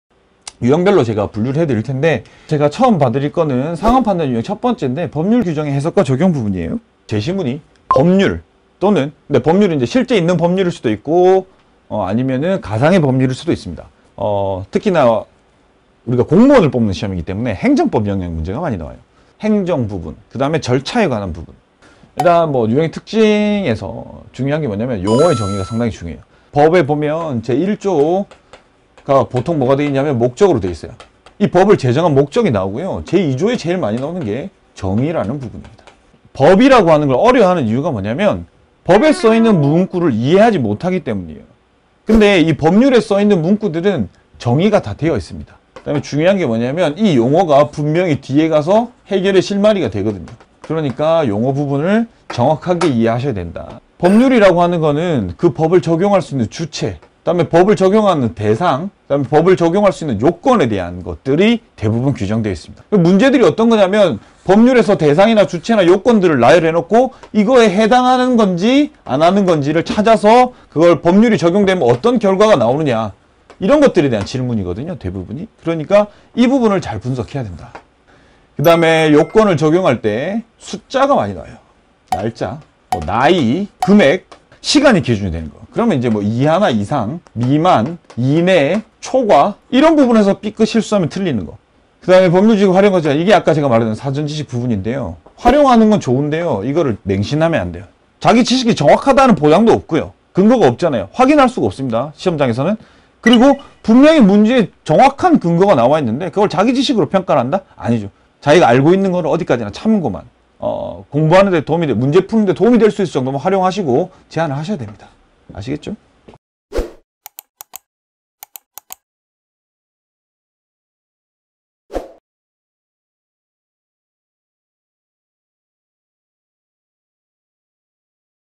[PSAT 공부법] 상황판단 법률문제 유형 특징 4가지/풀이팁!
7급공채 국가직 공무원&대통령경호처 PSAT 대비를 위한 3분완성 문제풀이!
공무원 7급PSAT 3분 안에 해결하자!
상황판단 법률규정 문제 유형 특징&풀이 TIP 대방출!
기출문제를 통해 실전에서 빠르게 풀 수 있는 진짜 "감각적 스킬"을 알려드립니다!

▶대통령경호처 7급 경호공무원 정보공유 오픈카톡방 바로가기👇